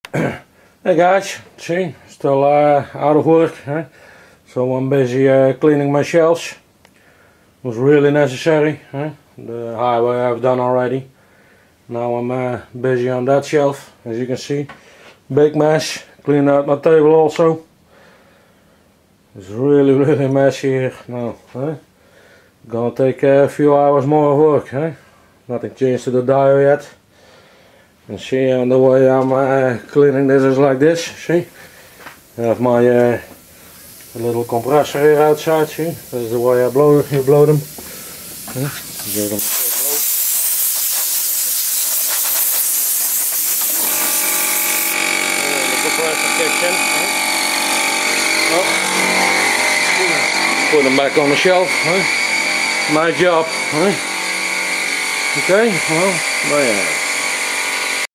Cleaning the models and shelfs today.
Todays job was cleaning all the models and shelfs. Also on the centerpice.
They sure do collect a lot off dust...